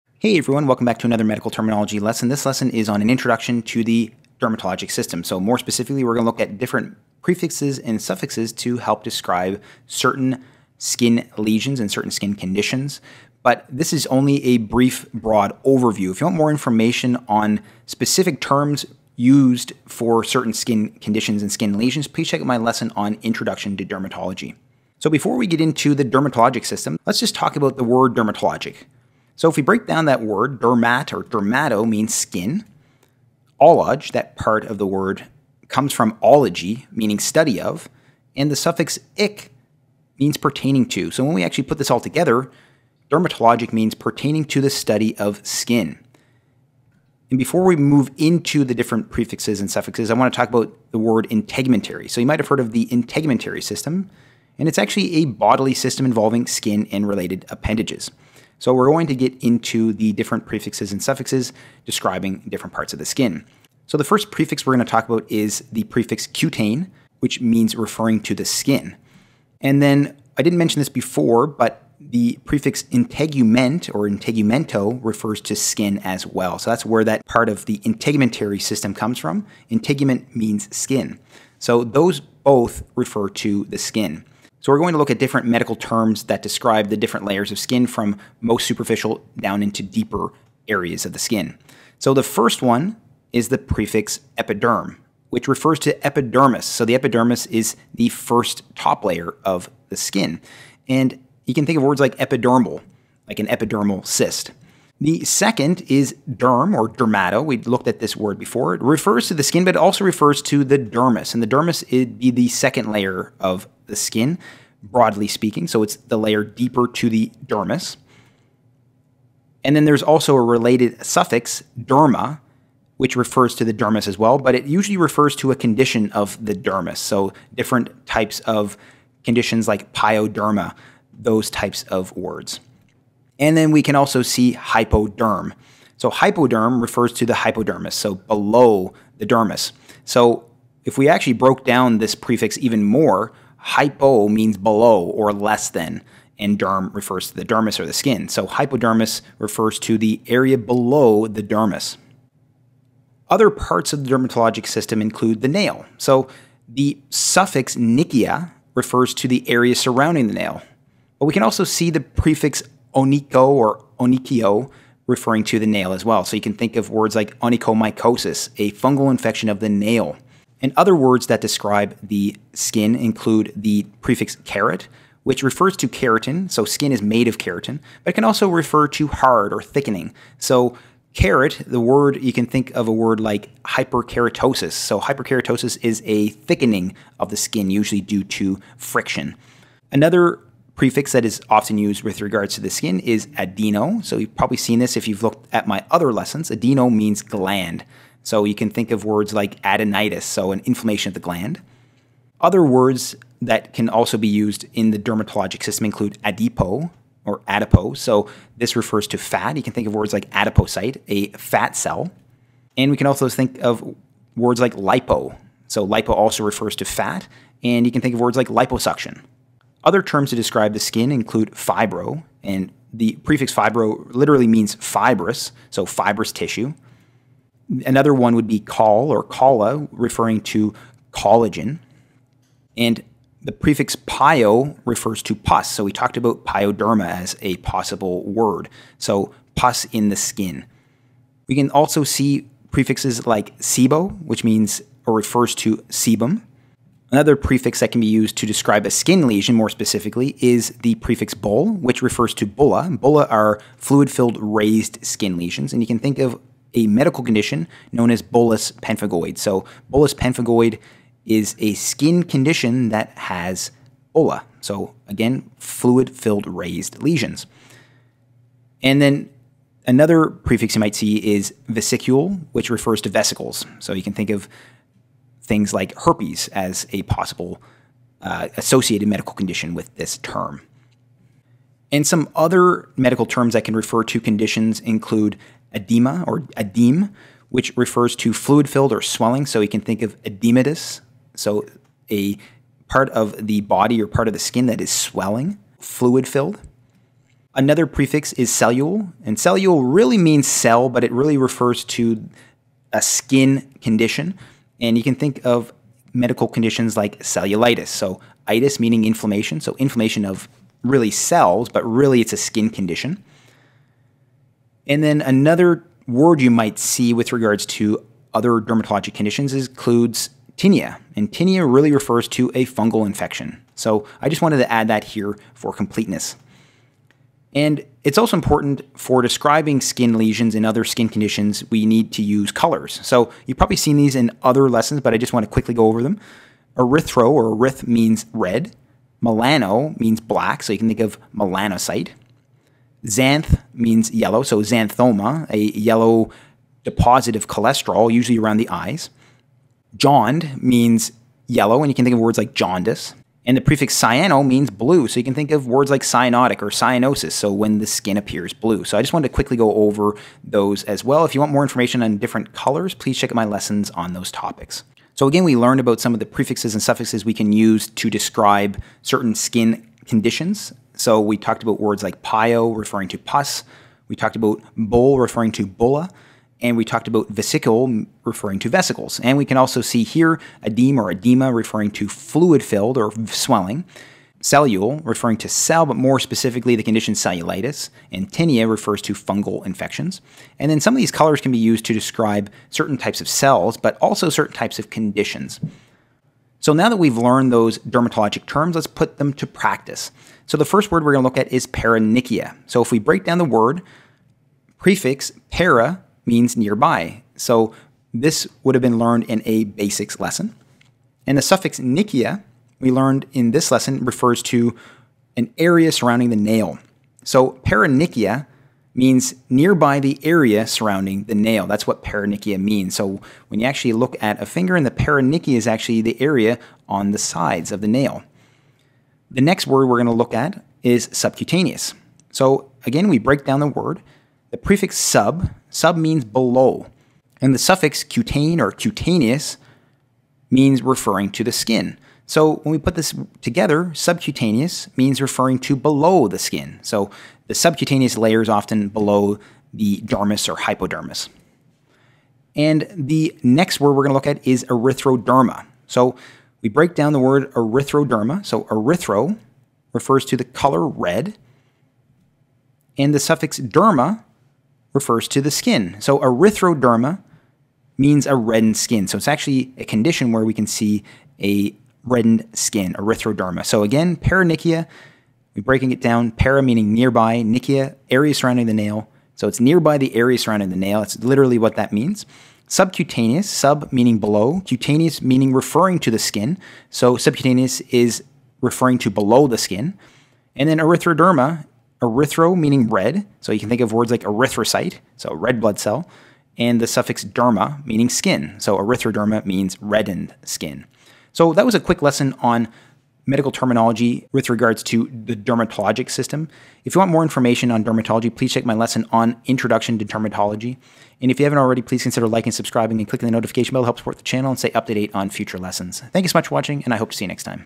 Medical Terminology | Lesson 10 | Skin and Skin Conditions (Dermatology)
Medical Terminology – Anatomy, Lesson 10: Dermatology (Skin and Skin Conditions). In this lesson, we discuss prefixes and suffixes describing the skin and related structures, skin anatomy and skin conditions.

JJ

*FOR BOOKS ON THIS TOPIC*
Amazon Store
Best Way to Remember and Memorize Medical Terms
Medical Terminology For Dummies
Medical Terminology Flash Cards